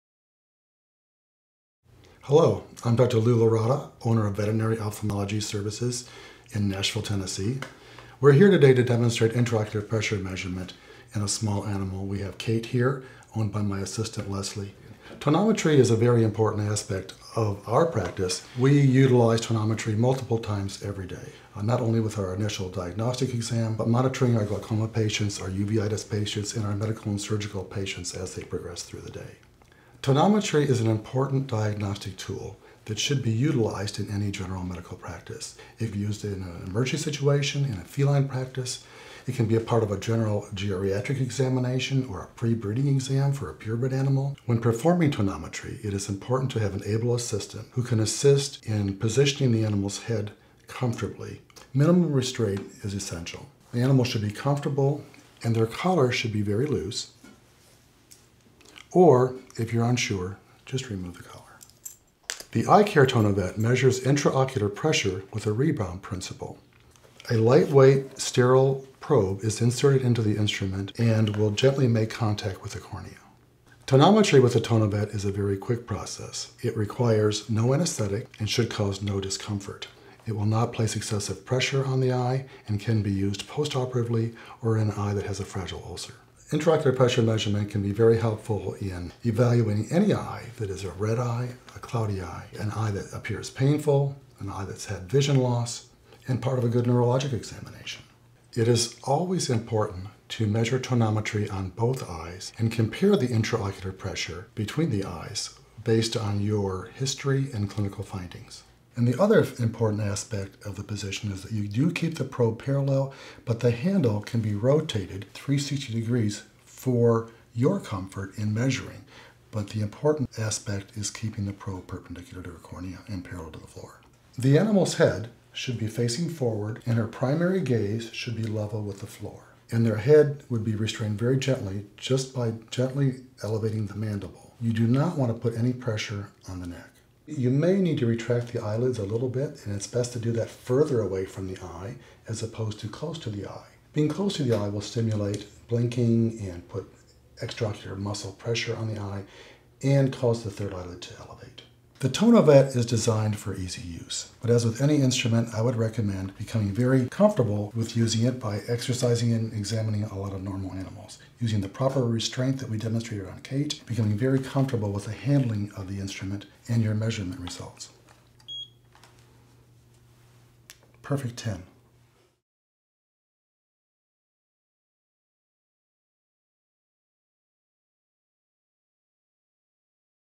Louis Laratta, DVM, DACVO, measuring dogs' eye pressure using the TONOVET tonometer
Dr Louis Laratta, Diplomate of ACVO, shows how to measure canine IOP (intra-ocular pressure), and explaines why and when it should be done.